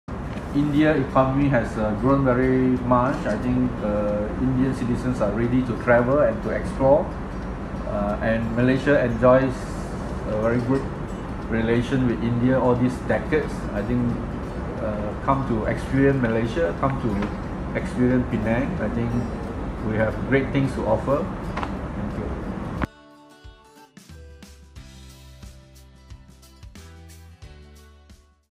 Chow Kon Yeow at PITE 2018
Chief Minister of Penang, Chow Kon Yeow speaks to Naina Nath, Head of Partnerships, Destination Reporter.